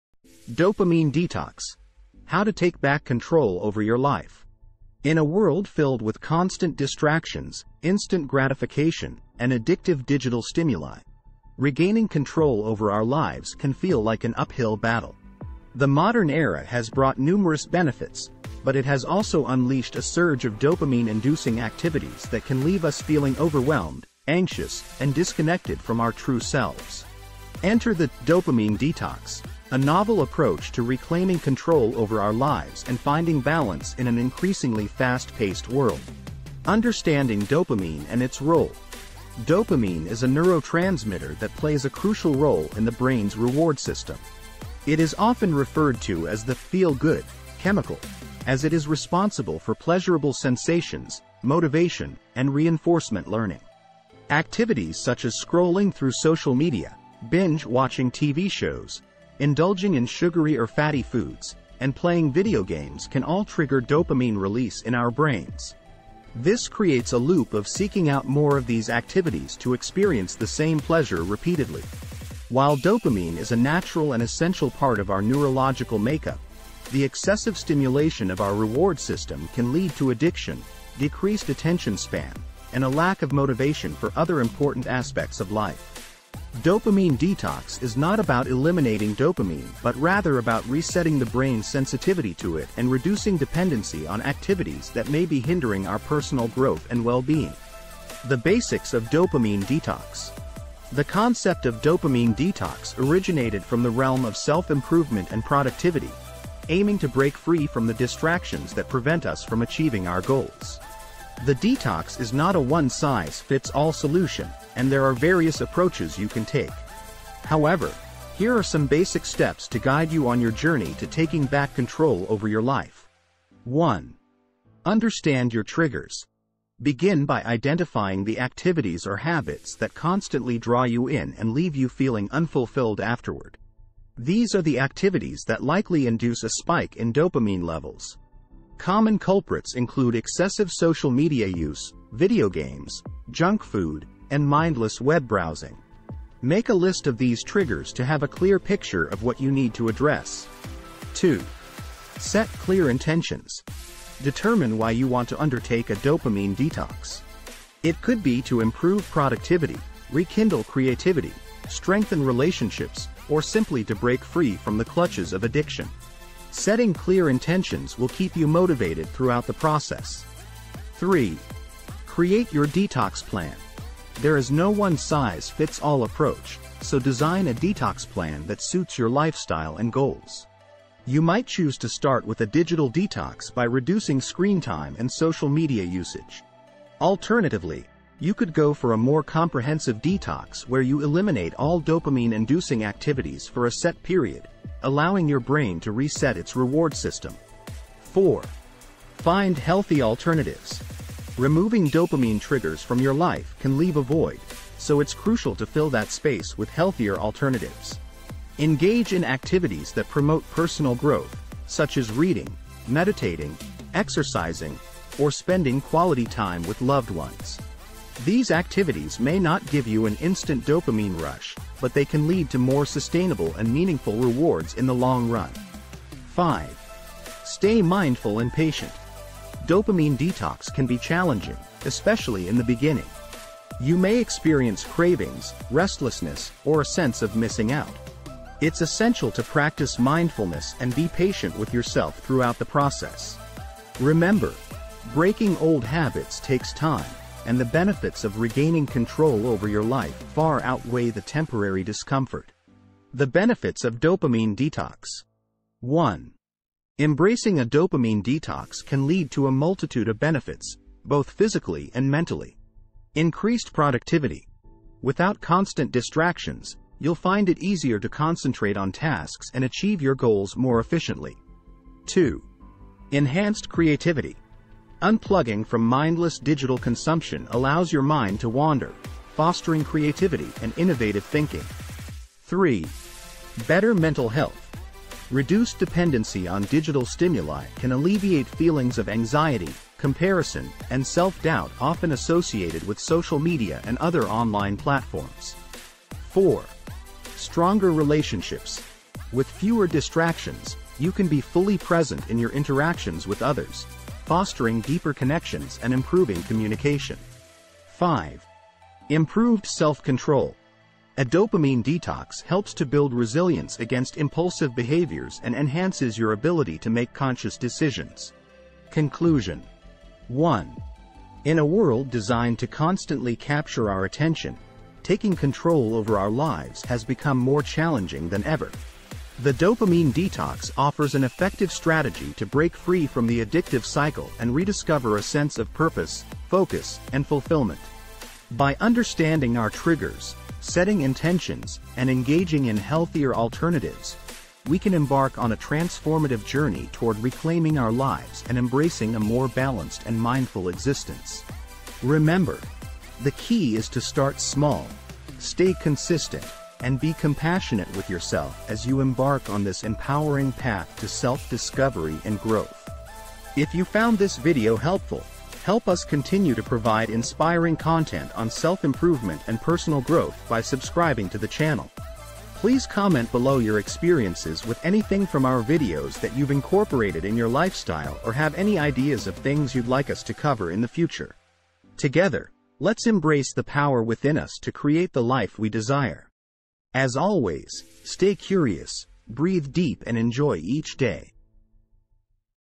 DOPAMINE DETOX: How To Take Back Control Over Your Life
In this fast-paced world of constant distractions, it's easy to lose sight of our goals and become overwhelmed by addictive behaviors. Join us on a journey as we explore the concept of Dopamine Detox, a powerful strategy to break free from digital distractions, regain focus, and find balance in our lives.